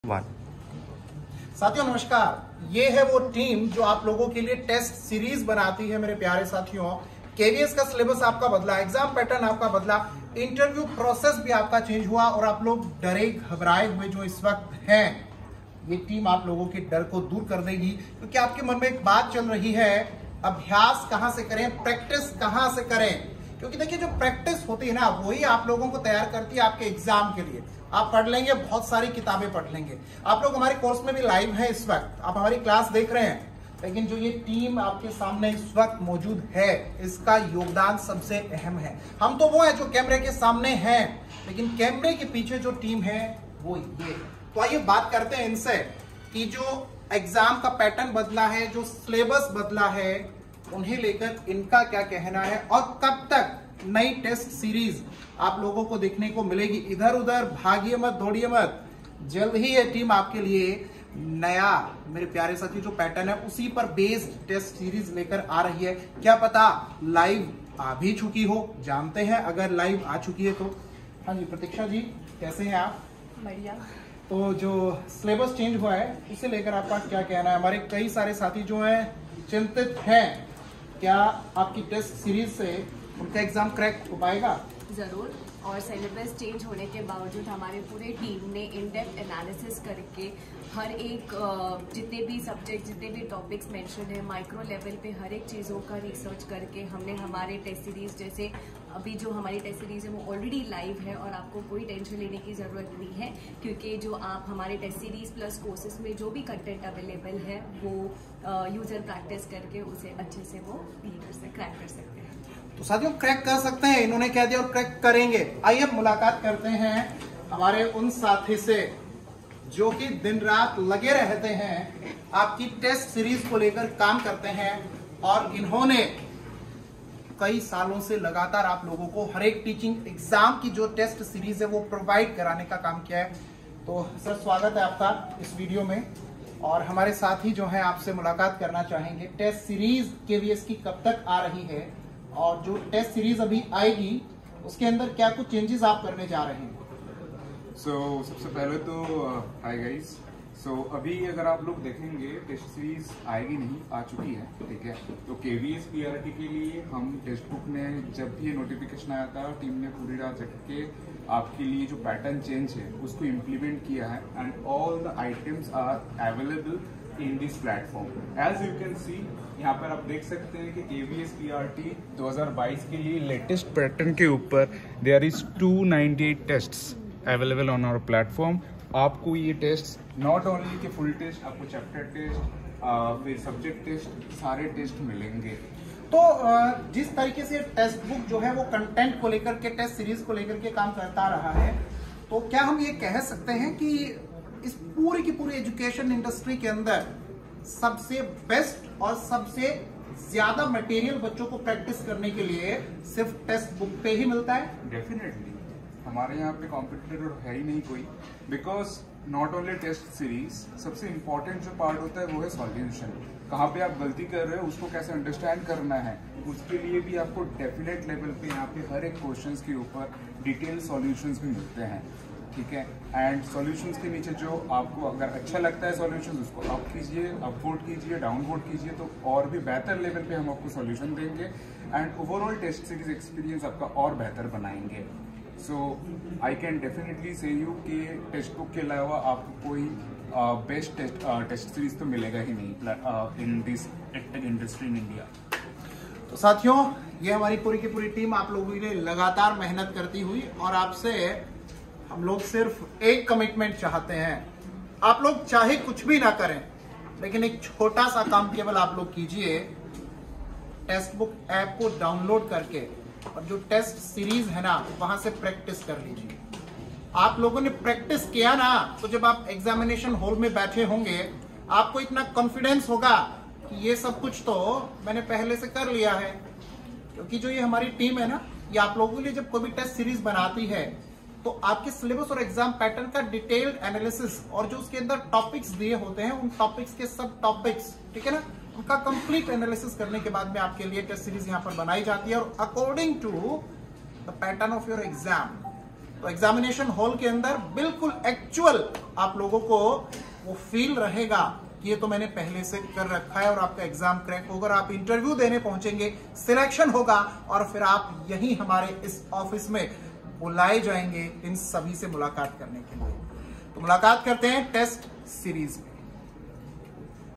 KVS 2022-23 PRT/TGT/PGT | New Syllabus वाली Test Series बना रही है | by Ajay Sir
If you're looking for a new syllabus for the KVS 2022-23 PRT/TGT/PGT  exam, then you're in luck! In this video, I'm going to show you the new syllabus for the KVS 2022-23 PRT/TGT/PGT  exam, which will be released in the fall semester of 2022-23.

This new syllabus is loaded with practice questions, and will help you prepare for the KVS 2022-23 PRT/TGT/PGT  exam with ease. So make sure to check out this video, and prepare for the KVS 2022-23 PRT/TGT/PGT  exam with the latest syllabus information!